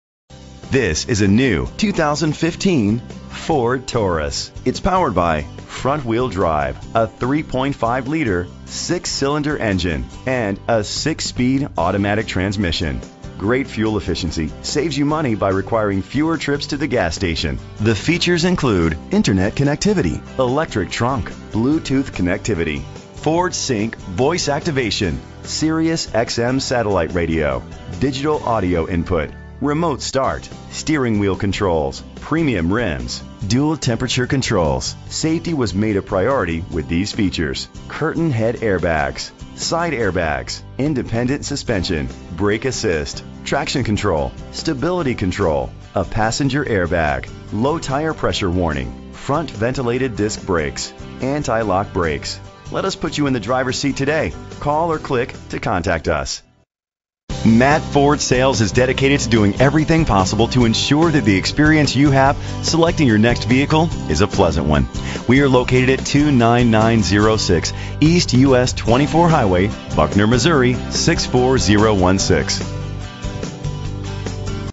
2015 Ford Taurus - Buckner MO
Phone:
Year: 2015
Make: Ford
Model: Taurus
Trim: SEL
Engine: 3.5 liter 6 cylinder 24 valve
Transmission: 6-Speed Automatic
Color: Magnetic Metallic
Mileage: 0
Address:
29906 E. U.S. 24 Hwy.
Buckner, MO 64016